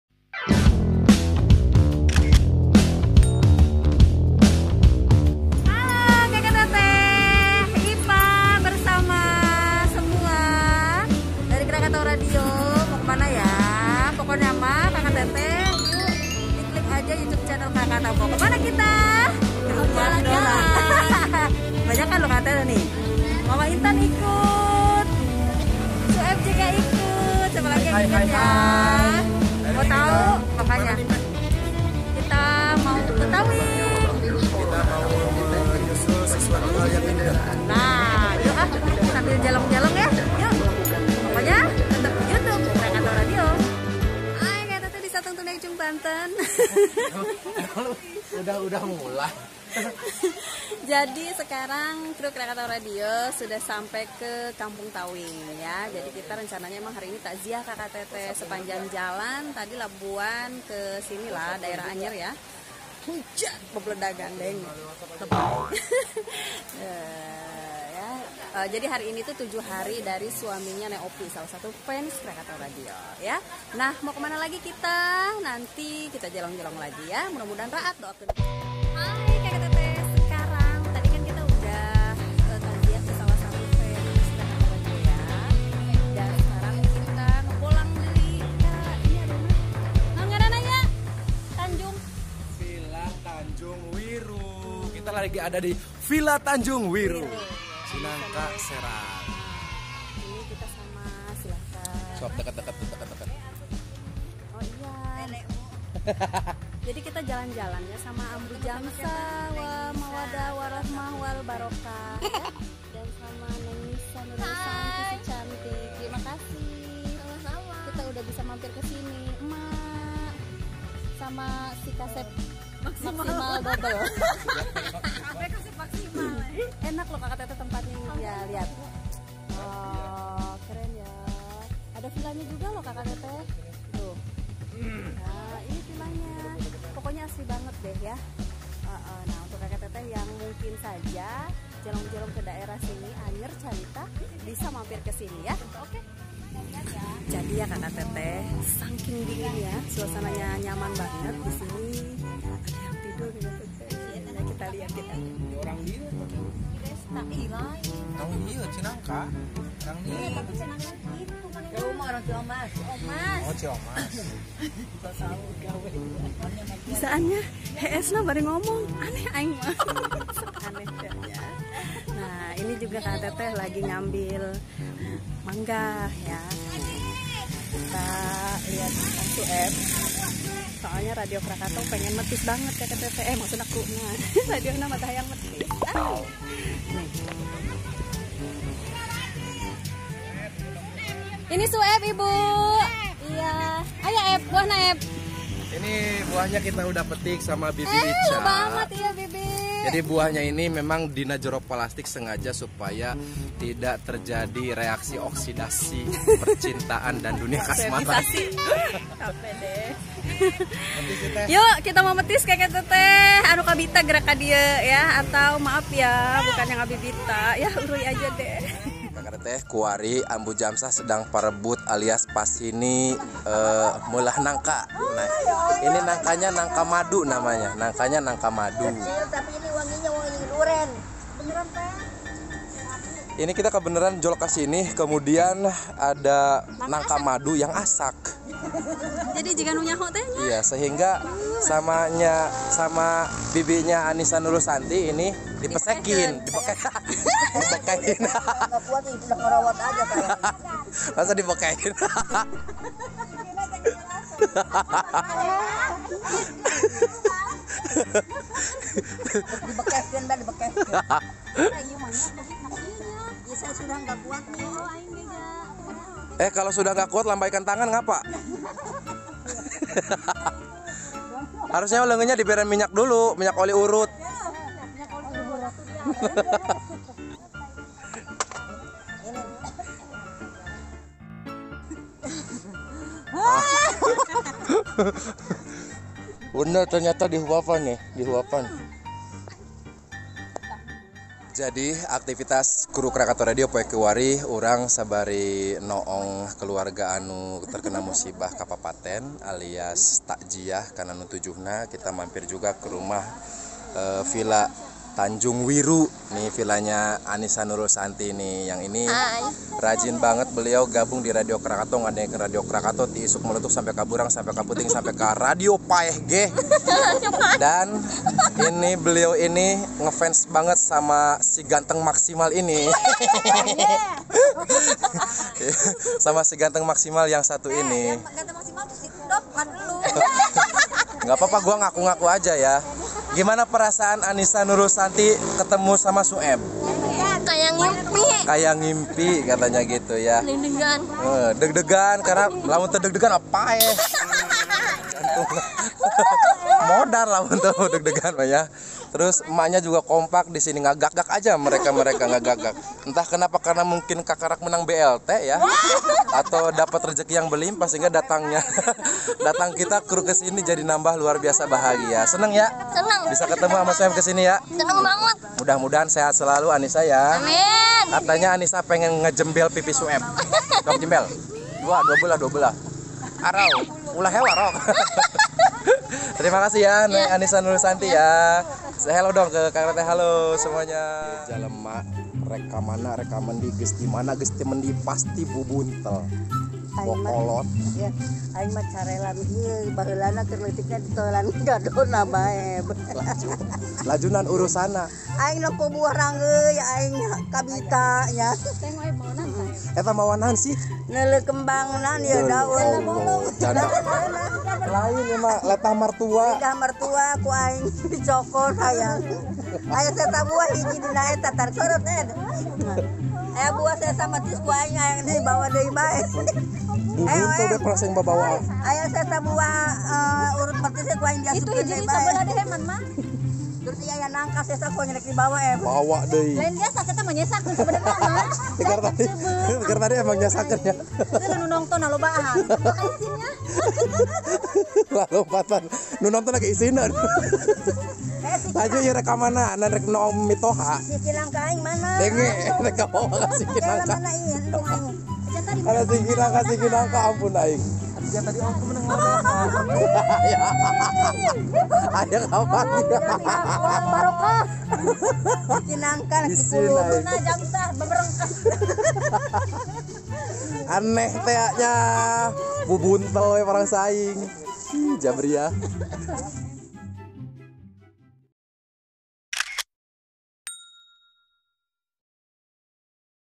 Krakatau Jalan-jalan: Ambu Jamsah Carelam, Bubuntel Bae
Halo Kaka Teteh, semua kru Krakatau Radio jalan-jalan nih ke Kecamatan Cinangka, Kabupaten Serang, Banten.

Kita takjiah ke salah satu fans radio dan mampir ke salah satu villa di Cinangka.

Seperti biasa, Ambu Jamsah selalu tampil kalau mau pulang. Aneh tea nyaaaah!?